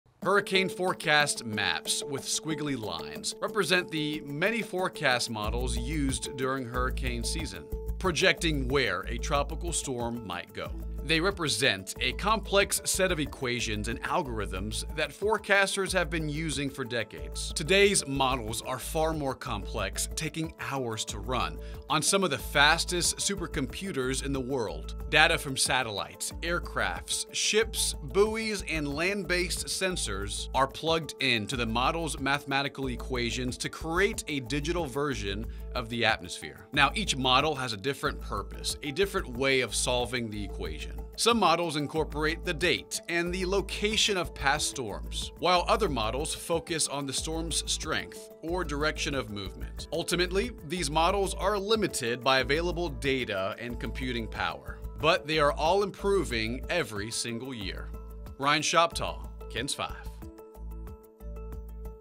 How do spaghetti models work in hurricane forecasting?
The hurricane center has been utilizing numerous forecast models also known as the spaghetti models. How do they work? Why are they important?